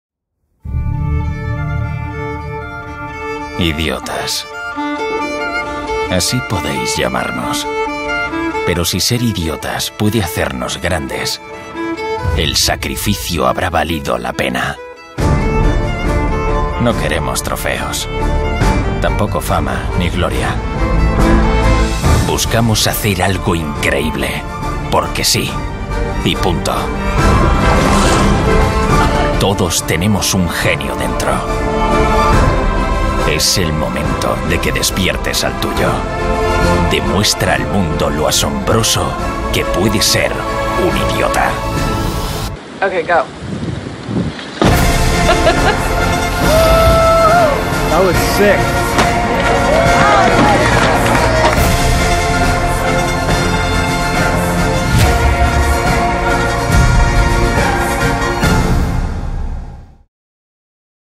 Fanta , Nueva Campaña "In the name of the play", spot 60
La campaña arrancó el pasado 27 de febrero con una pieza teaser de 20”. A partir de hoy, se complementa con la emisión en televisión de un spot de 30” que protagonizan las mismas personas que, en su día, arrasaron en Internet subiendo sus vídeos, que se hicieron virales por contenido lúdico.

Se trata de hacer un llamamiento a todos para disfrutar de la diversión espontánea y desestresante. Se complementará con piezas de 60” y 20”; y para los formatos digitales se utilizarán piezas de 15” y 6”,